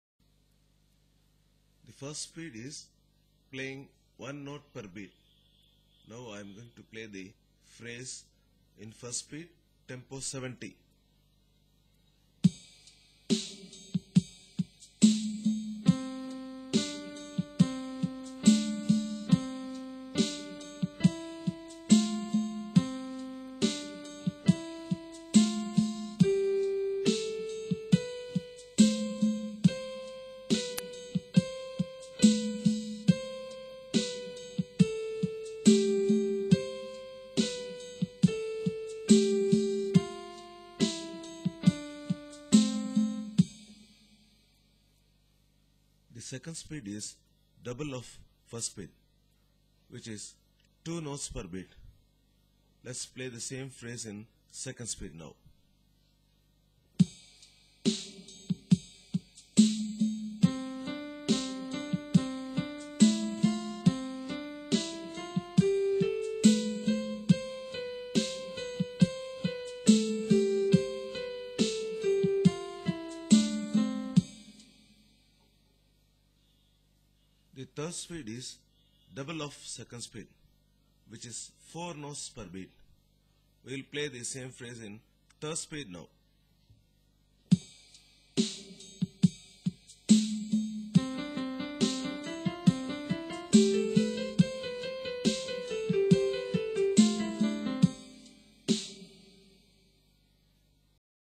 Different Speeds in Music
This is about different speeds in music, and how notes are played in different speeds..For learning music/ keyboard online, please